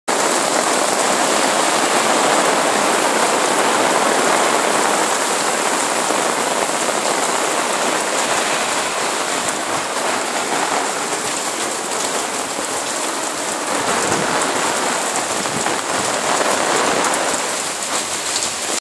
Pescara temporale 10 giugno 2013
Pescara: il temporale si scatena e la grandine imbianca le strade